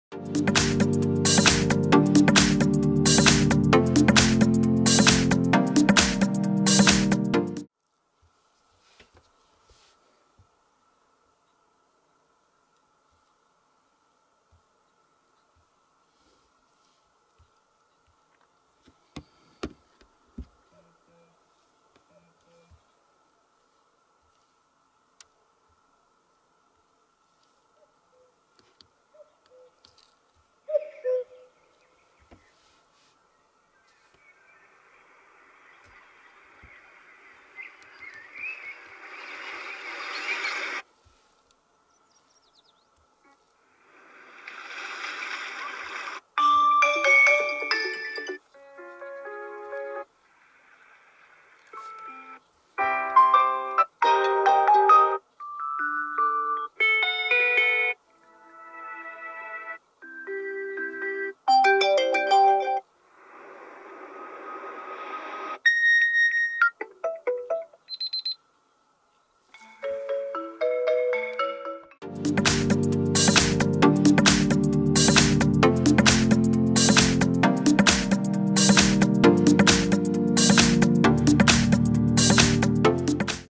All alarm tones on Huawei P50 Pocket / default alarm tones on Huawei P50 Pocket
Welcome back, in today's video our expert will show all the possible tones of alarms. Today's device will be Huawei P50 Pocket.

How to turn on the alarm on Huawei P50 Pocket phone? Where to set the alarm on Huawei P50 Pocket? What alarms are available on Huawei P50 Pocket?